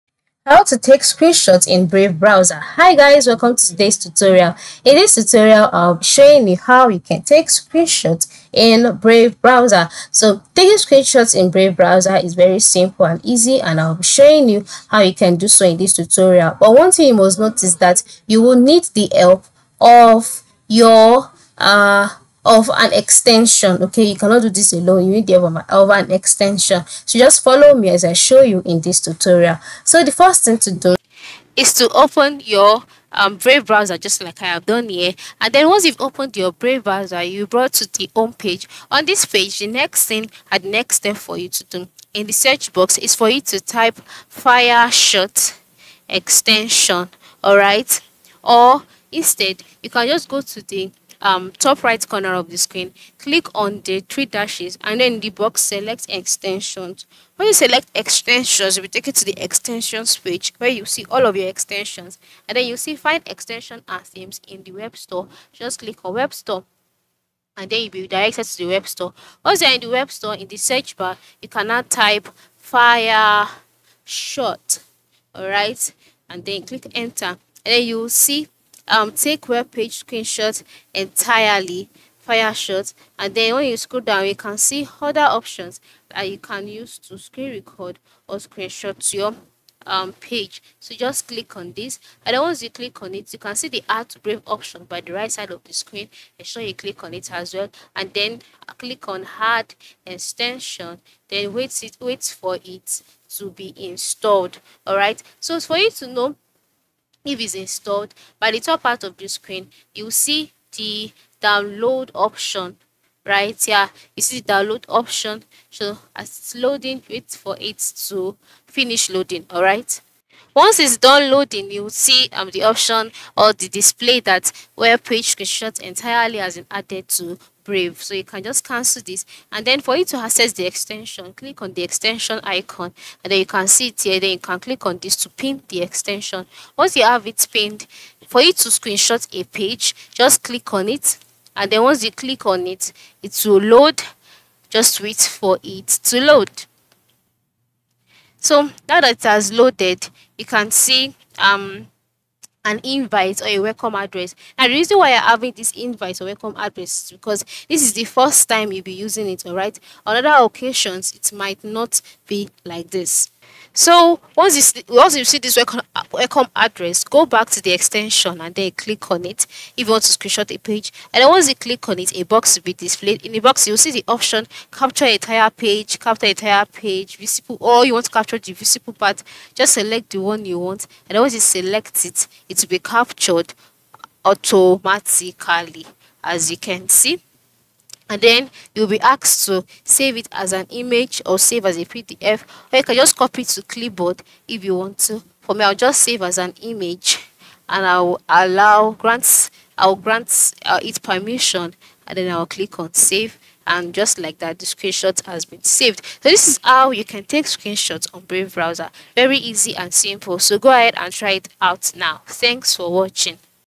How To Take Screenshots In Brave Browser (Easiest Way)​
Today we talk about How To Take Screenshots In Brave Browser, so stay until the end of the video to see the full explanation.